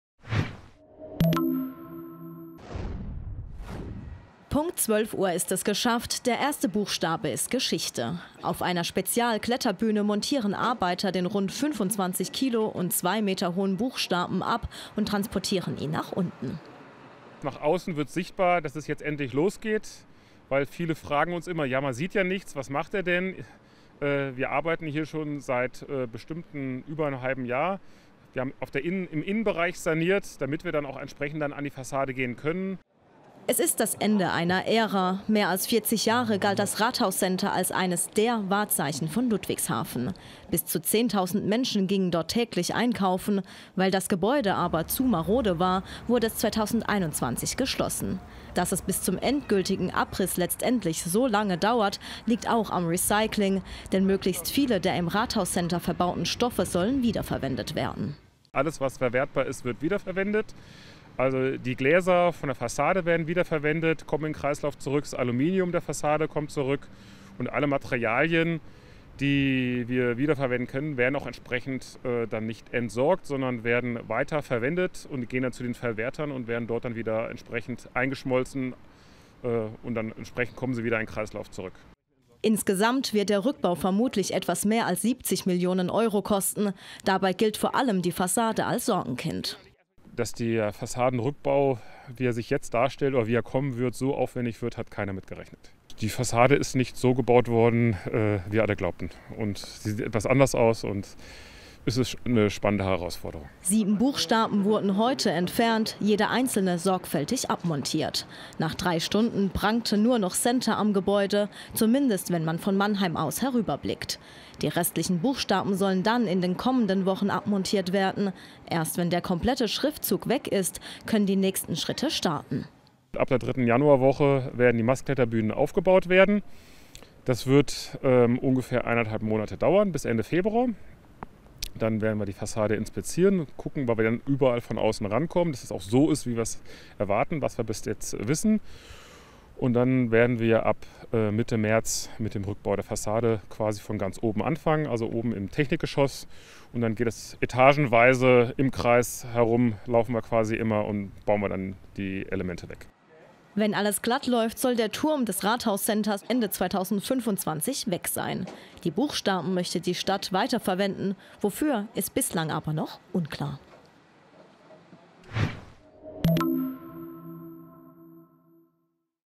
Aus „Rathauscenter“ wird „Center“ | RON TV
Abbau beginnt:
In Ludwigshafen haben heute die ersten Abbauarbeiten des Rathauscenters begonnen. Ganze sieben Buchstaben wurden abmontiert. Nun ist der ikonische Schriftzug, der über 40 Jahre das Bild der Stadt geprägt hat, Geschichte. Anfang 2026 soll das ganze Gebäude abgerissen werden. Wir waren bei dem Abbau dabei.